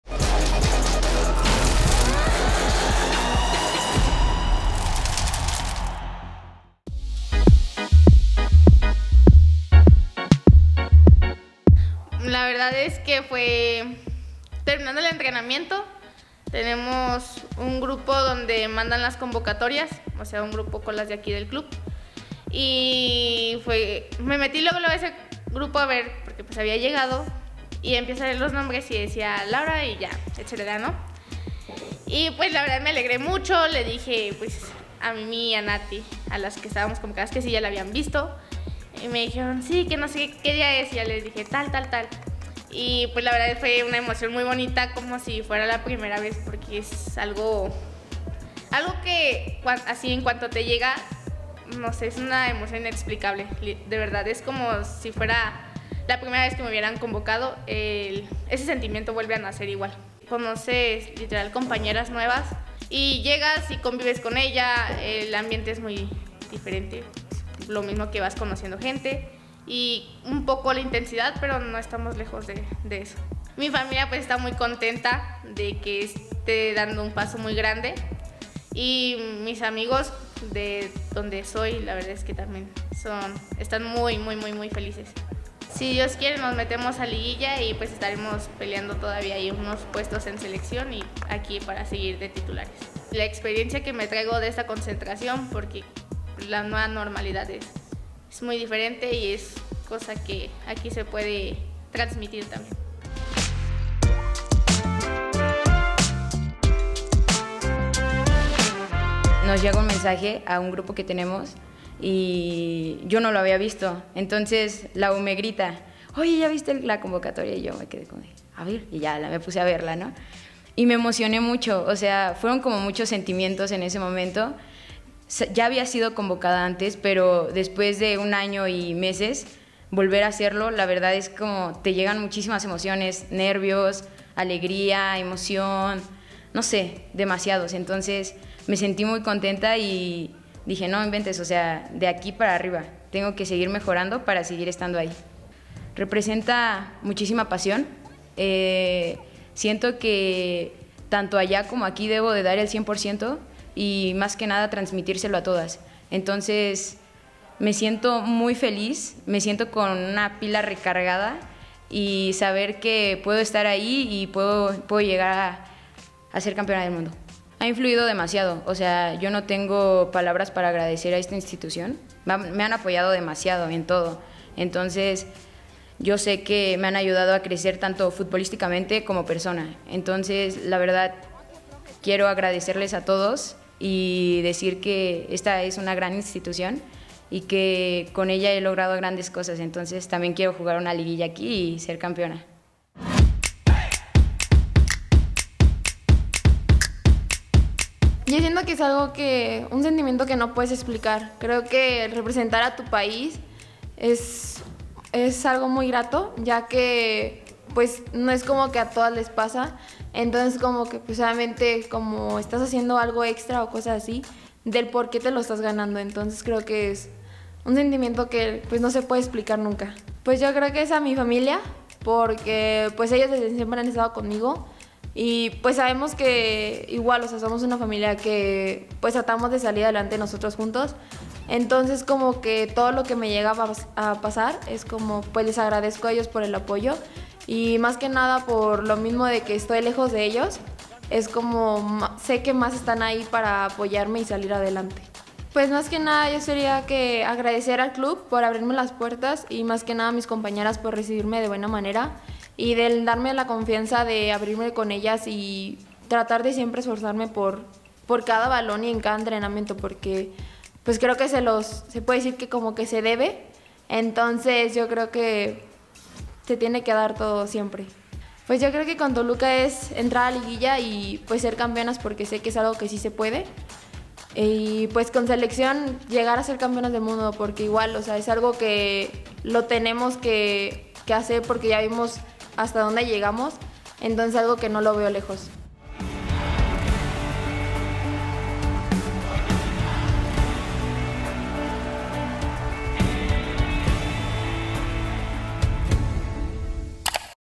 Diablas de Selección
Natalia Mauléon, Laura Parra y Noemí Granados nos contaron su experiencia respecto a su última convocatoria con selección mexicana femenil sub-20.